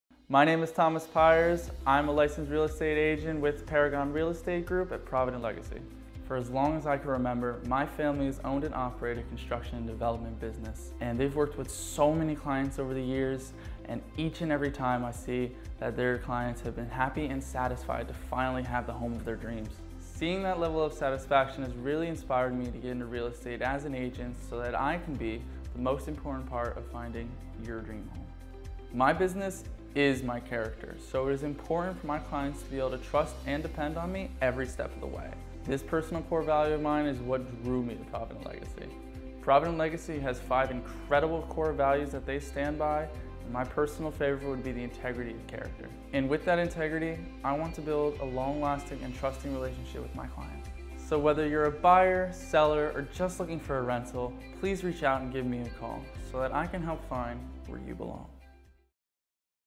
Thomas Pires Agent Intro Video
Provident Legacy Real Estate Agent Thomas Pires's promo video!